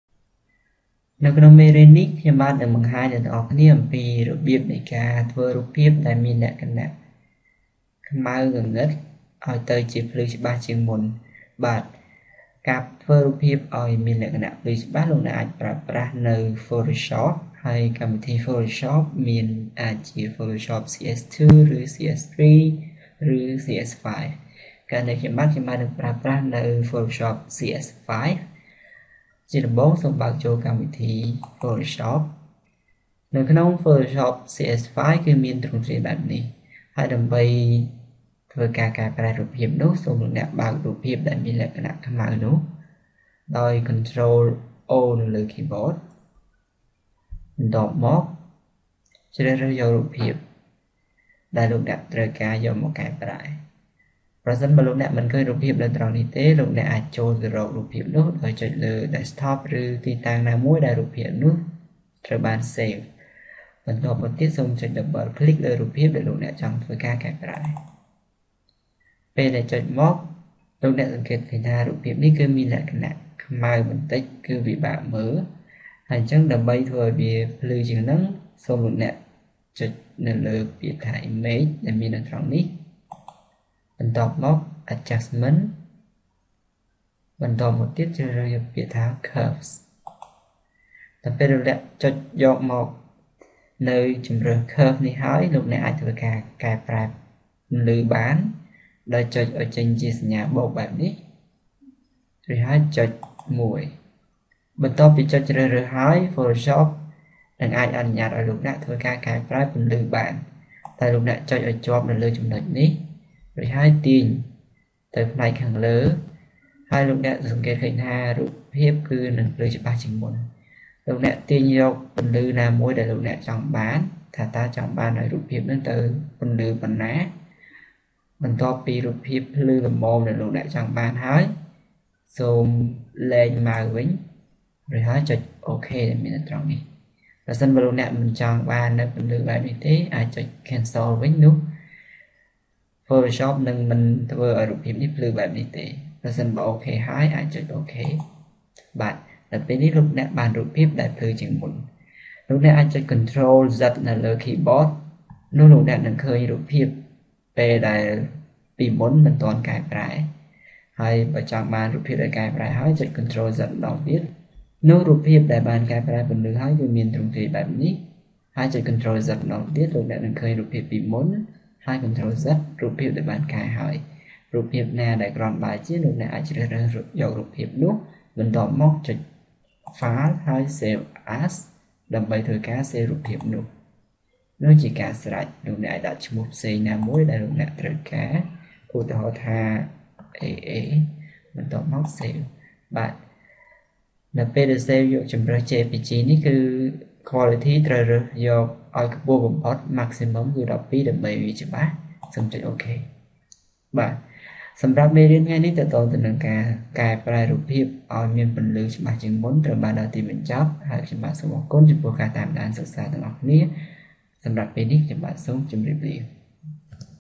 How to make dark picture lighter by using Photoshop
How to make dark picture lighter by using Photoshop with Khmer tutorial. Now learn how to design Photoshop with me easy way, and learn faster and faster, and also so much improve your knowledge.